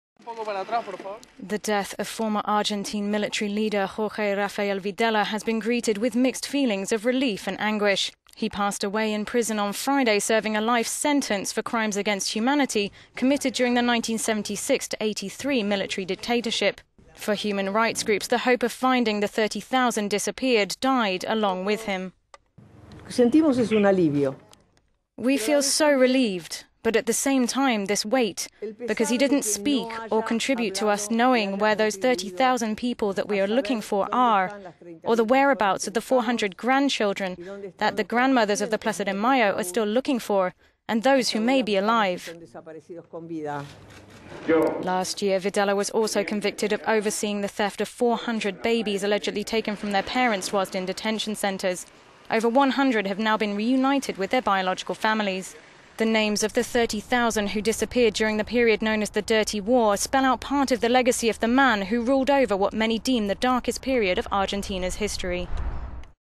Argentina: Ex-military leader Videla's death leaves unanswered questions for 'disappeared'
The death of former Argentine military leader Jorge Rafael Videla has been greeted with mixed feelings of relief and anguish.

He passed away in prison on Friday serving a life sentence for crimes against humanity committed during the 1976-83 military dictatorship.

For human rights groups the hope of finding the 30,000 'disappeared' died along with him.

President of the Grandmothers of the Plaza de Mayo, Estela de Carlotto said, "We feel relieved but at the same time this weight because he didn't speak or contribute to us knowing where those 30,000 people that we are looking for are or the whereabouts of the 400 grandchildren that the Grandmothers [of Plaza de Mayo] are still looking for."

Last year Videla was also convicted of overseeing the theft of 400 babies allegedly taken from their parents whilst in detention centres. Over 100 have now been reunited with their biological families.

The names of the 30,000 who disappeared during the period known as the 'Dirty War', spell out part of the legacy of the man who ruled over what many deem the darkest period of Argentina's history.